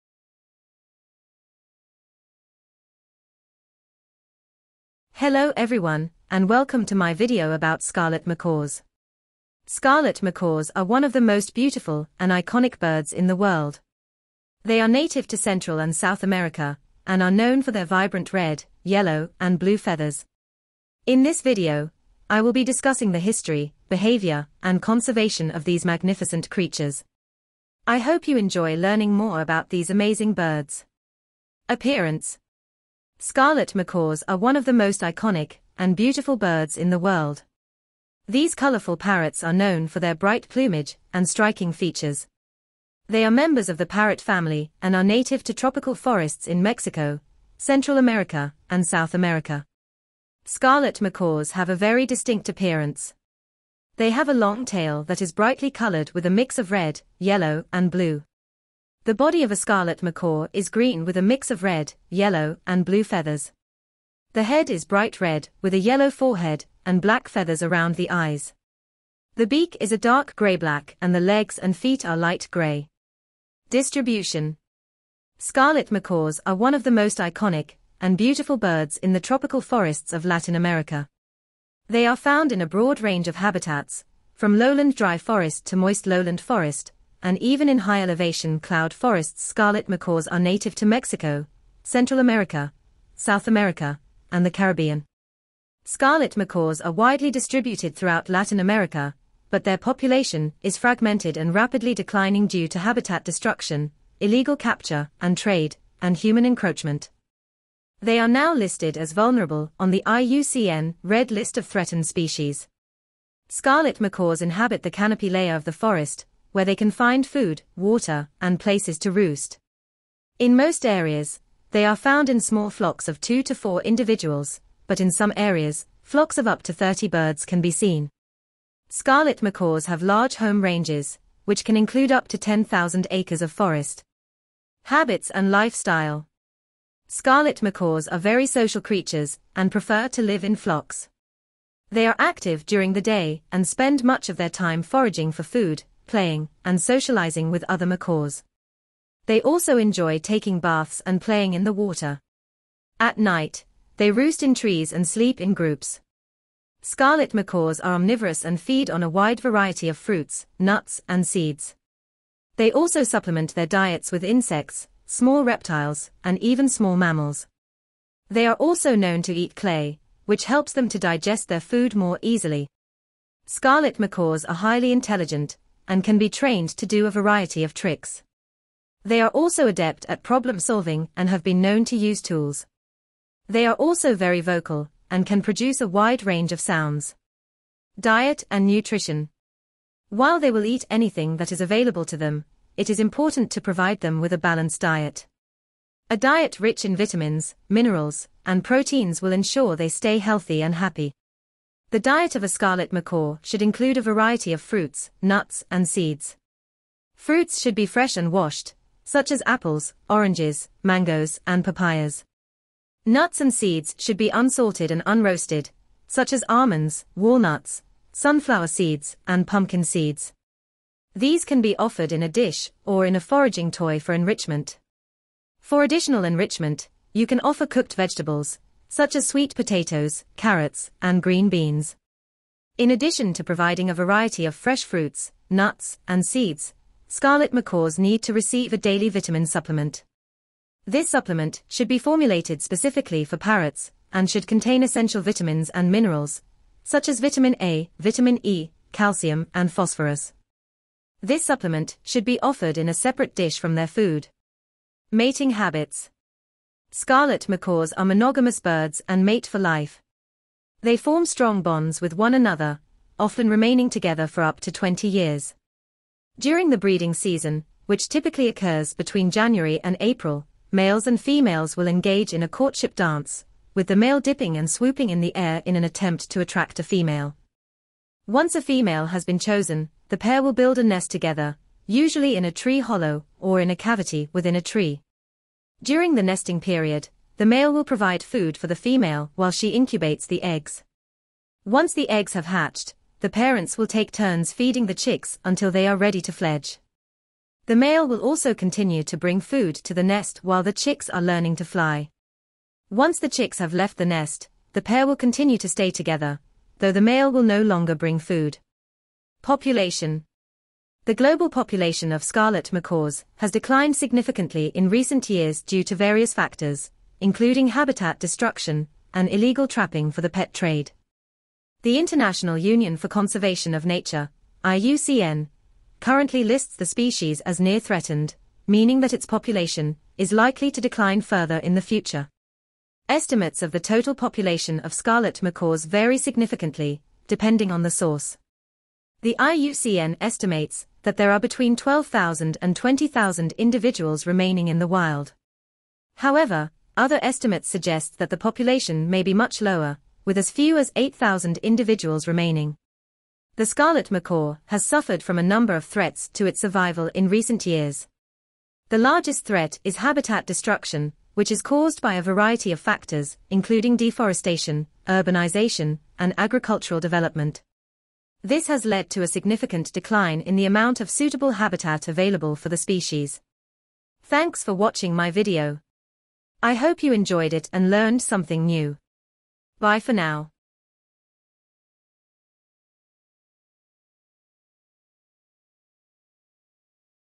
THE SCARLET MACAWS OF THE AMERICAS - WILDLIFE REDISCOVERED EP-2
Welcome to our video on Scarlet Macaws! These beautiful and vibrant birds are a delight to watch and are one of the most iconic birds of the rainforest. In this video, we will be exploring the stunning Scarlet Macaw. We will discuss their unique physical characteristics, their fascinating behavior, and their amazing array of vocalizations. We will also highlight the threats that Scarlet Macaws face in the wild and how we can help to ensure their survival. So join us as we take a closer look at these amazing birds!